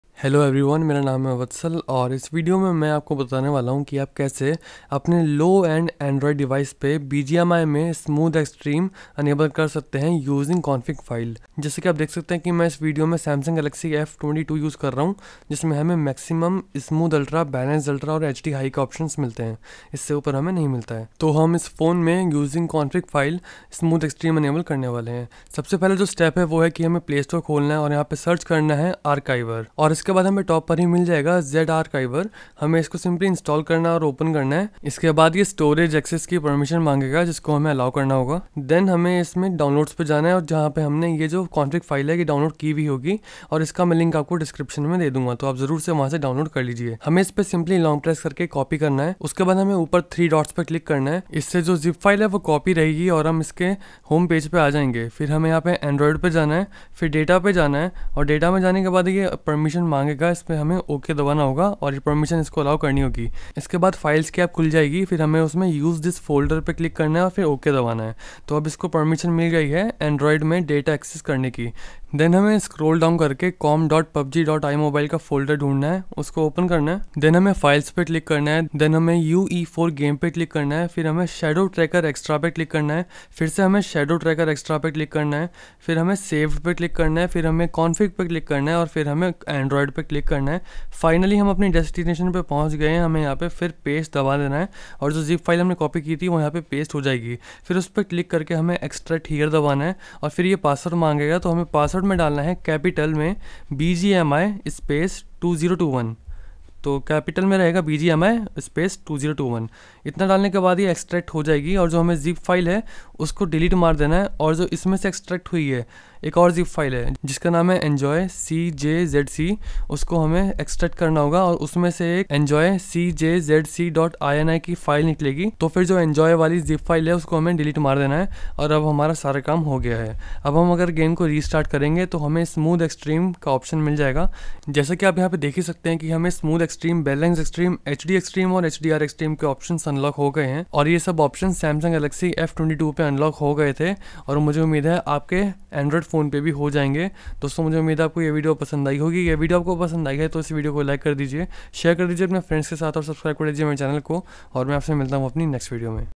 Enable 60 FPS Smooth+Extreme in BGMI on any Android Smartphone!
In this video, i will tell you How to enable 60 FPS Smooth+Extreme in BGMI on any Android Smartphone using Config File Method.

Help My Channel by using Below Links for Your ORDERS